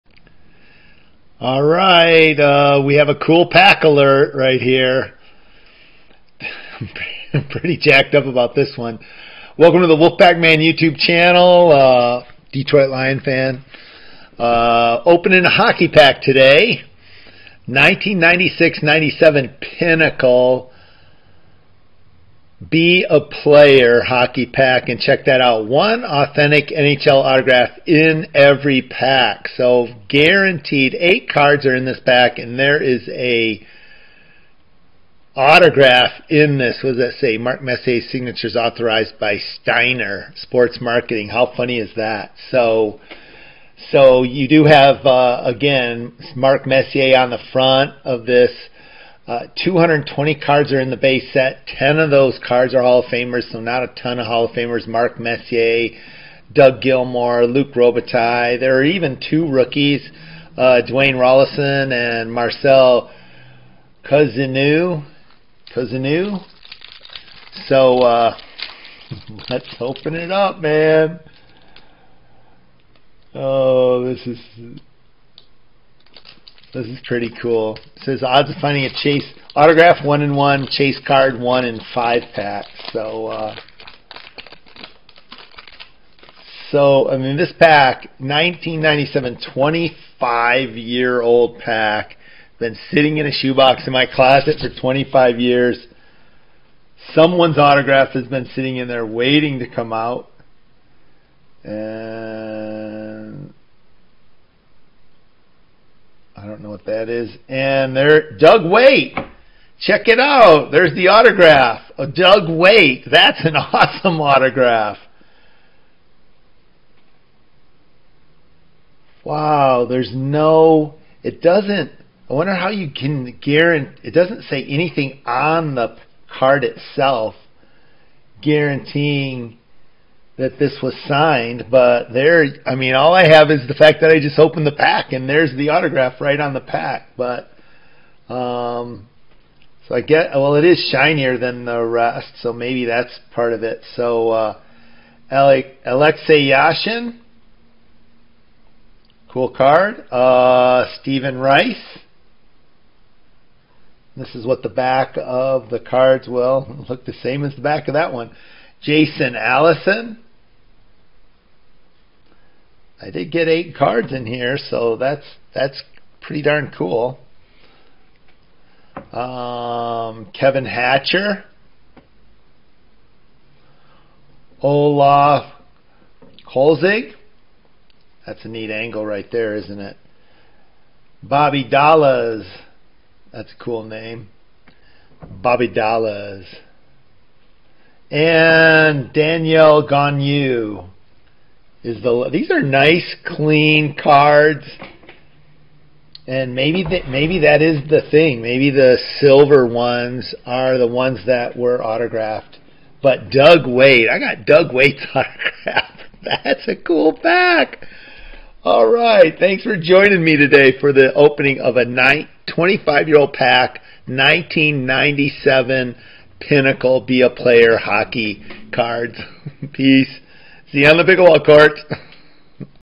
DOUG WEIGHT Autograph Wednesday #6 pack rip with 1996-97 Pinnacle BE A PLAYER hockey!
Welcome to HOCKEY AUTOGRAPH WEDNESDAYS
at the WOLFPACKMAN YouTube channel!

Today I'm opening a pack of 1996-97 Pinnacle Be A Player hockey cards!

8 cards in this hobby pack with Mark Messier
on the front AND an AUTOGRAPH IN THE PACK!

I paid $5 for this pack!

220 cards are in the base set including
10 HOFer cards with players like
Mark Messier,
Doug Gilmour,
Luc Robitaille.

2 rookie cards.
Dwayne Roloson,
Marcel Cousineau.

Found:
Doug Weight AUTOGRAPH plus
Kevin Hatcher,
Alexei Yashin,
Olaf Kolzig!